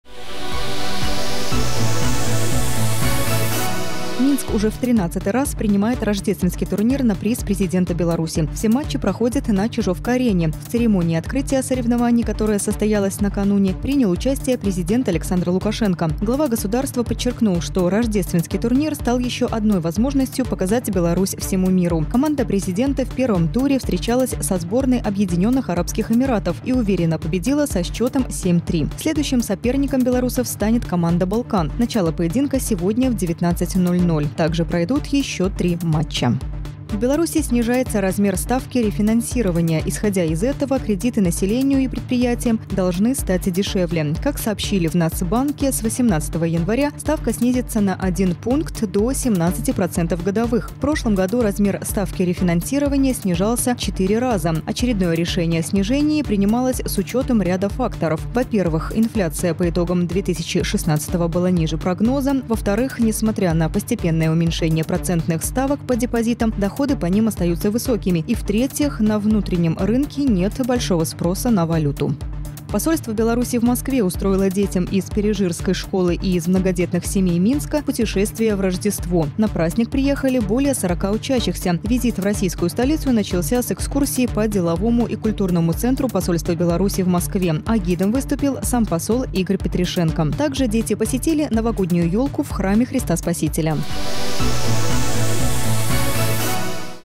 90 секунд, 5 января 2017 года, 10 00
В Минске продолжается XIII Рождественский турнир
 Размер ставки рефинансирования снижается
Подарок детям от Посольства Беларуси в России